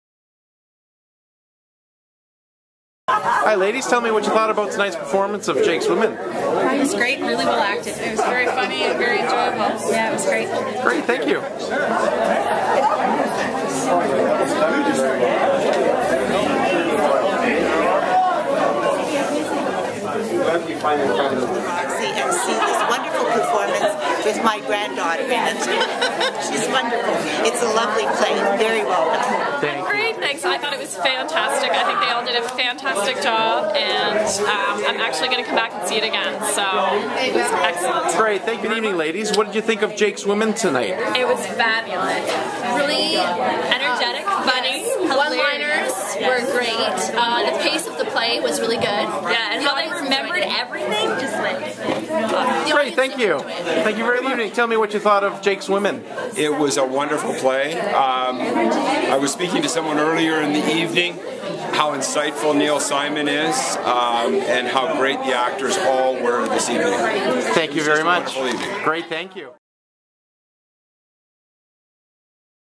The reviews are in: people love Jake's Women!
Audience reviews from the opening night of Jake's Women at Owen Sound's Roxy Theatre, June 2, 2011.